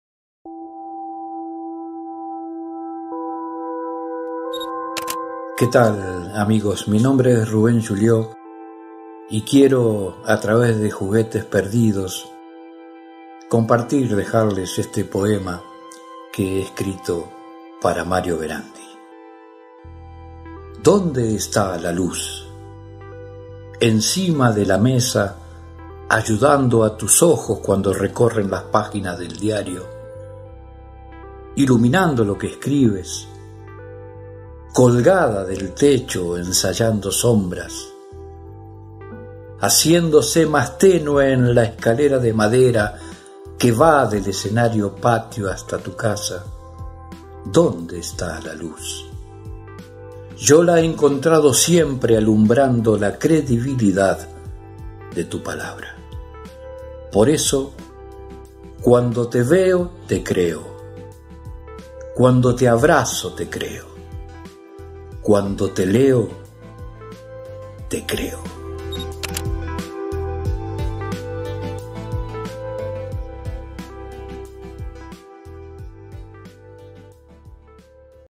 POEMA de Rubén Yulió
Poema del artista nicoleño Rubén Yulió dediado a Mario Verandi.